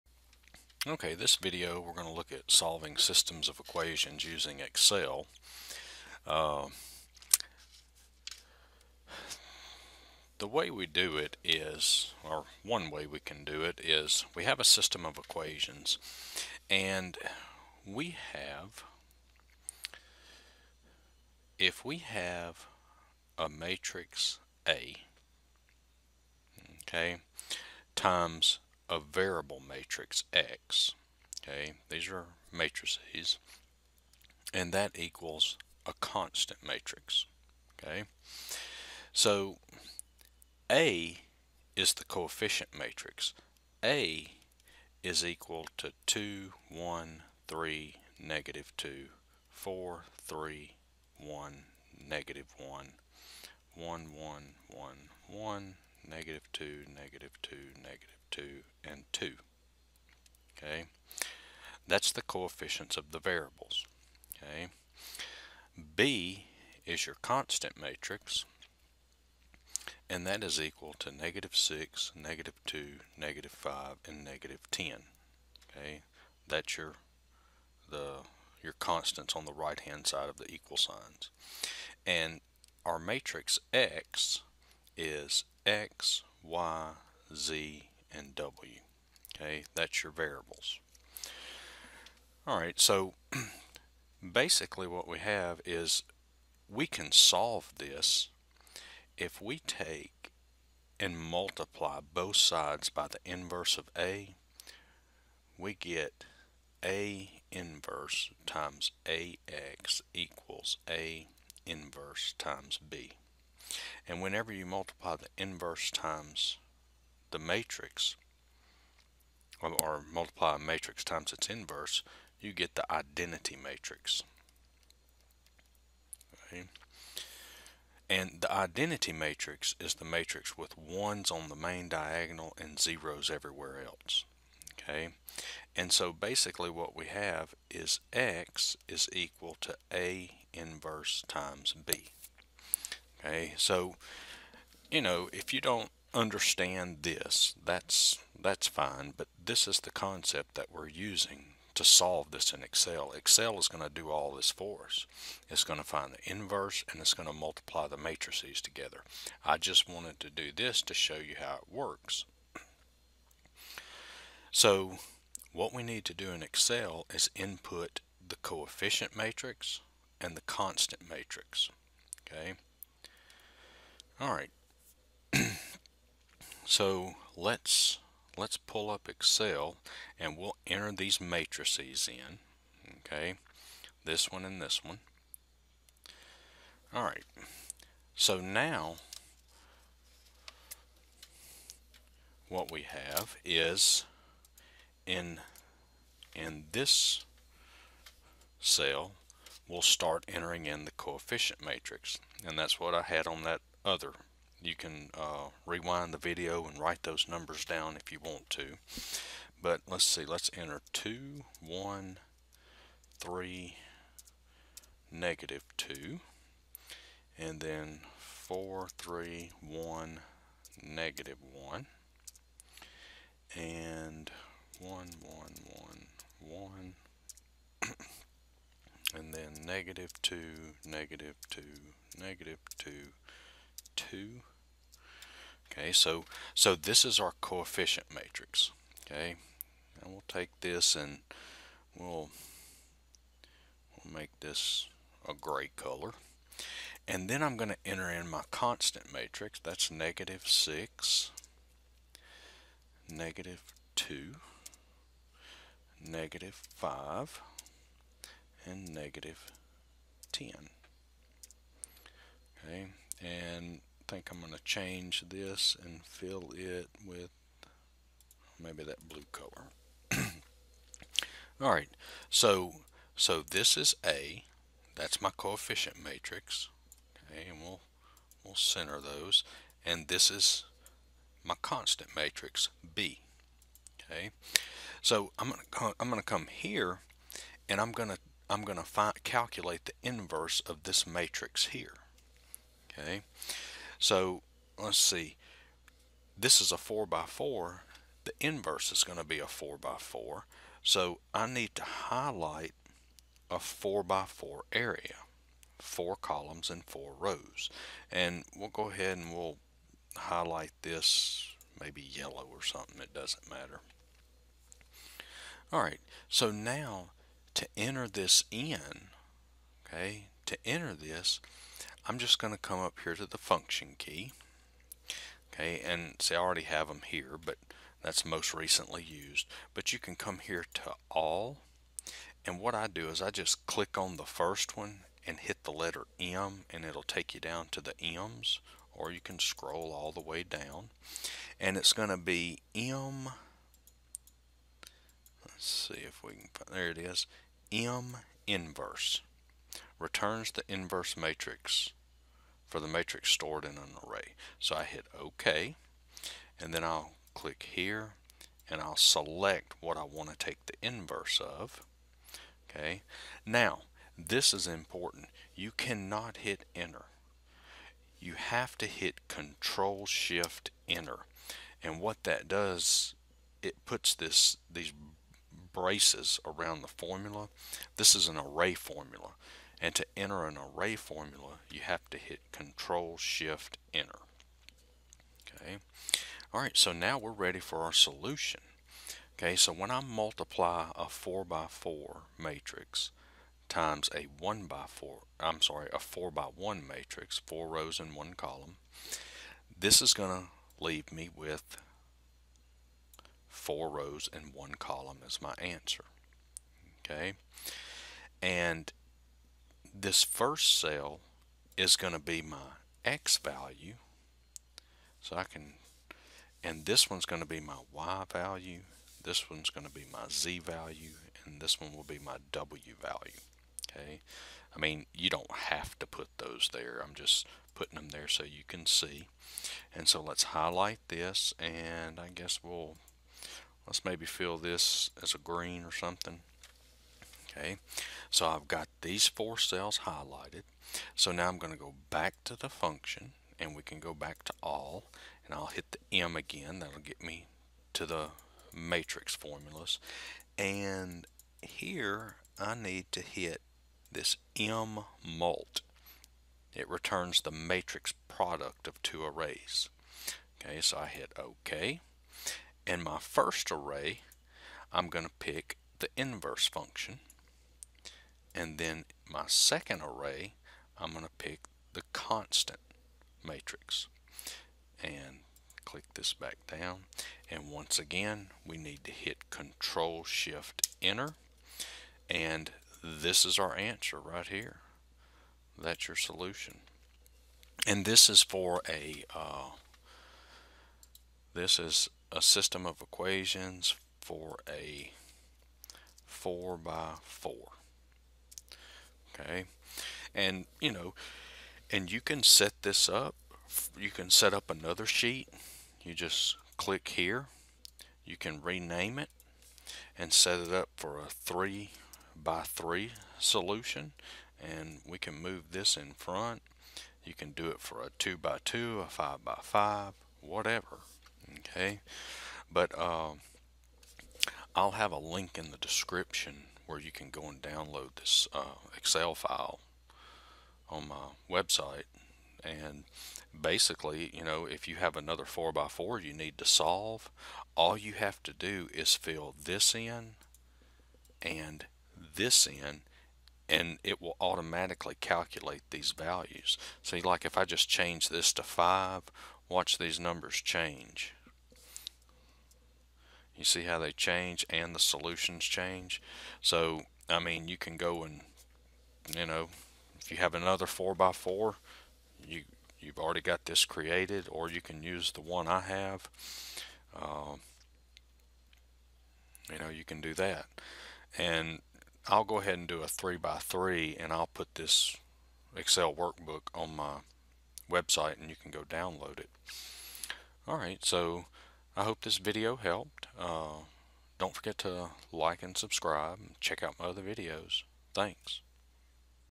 Solving System of Equations Using Excel
This video show how to solve a system of equations using Microsoft Excel.